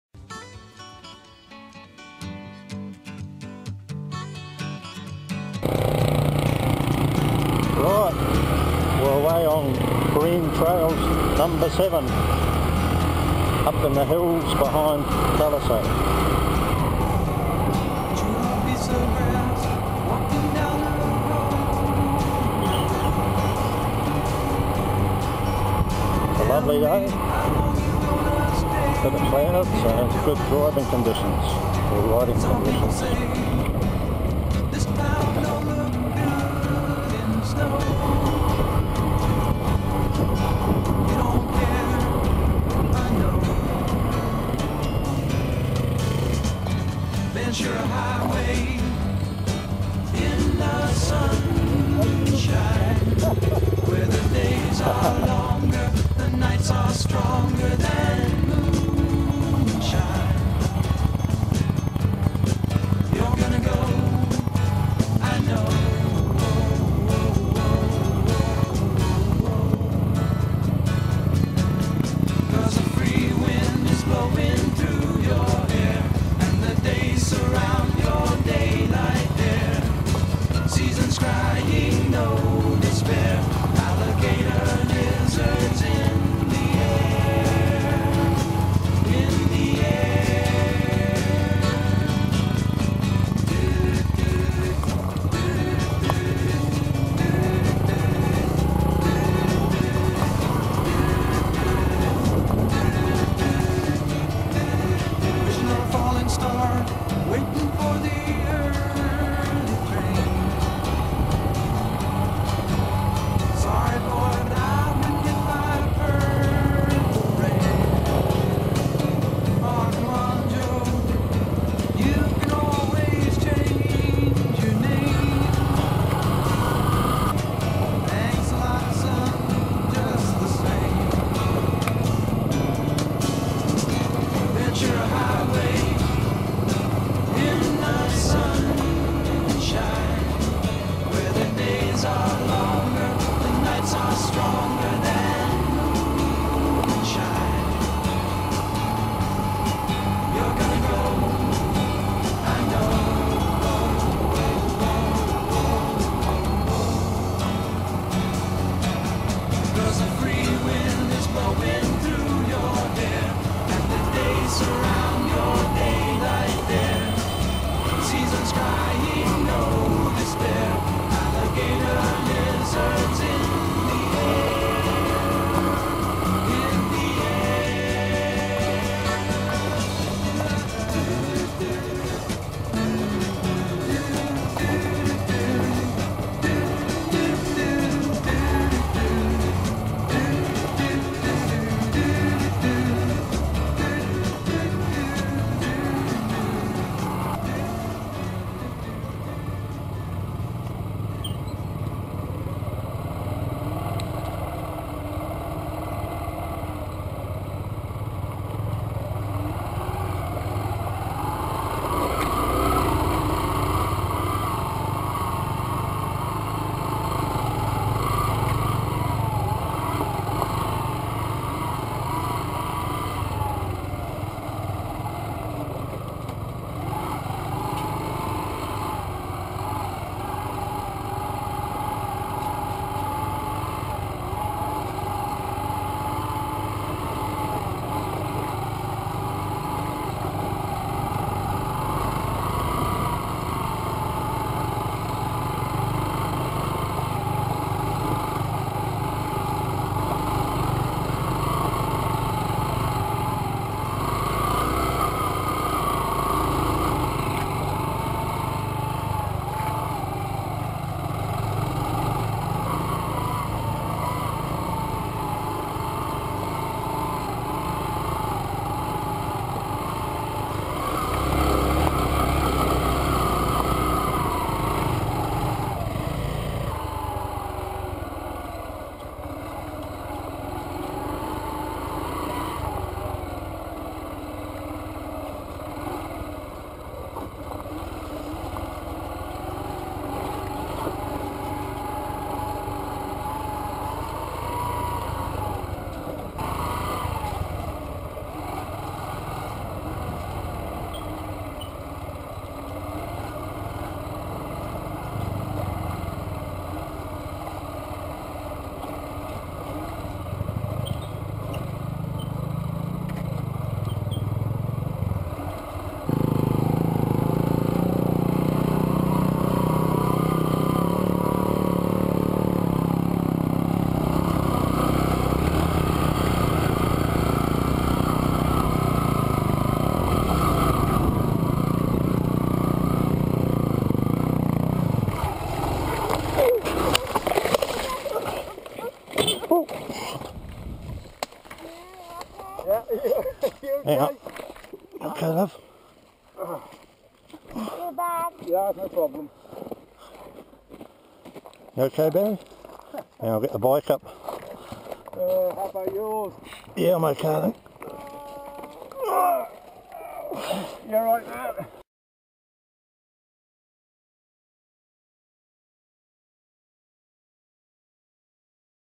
green trail # 7.wmv
motor bike ride in the hills behind talisay cebu philippines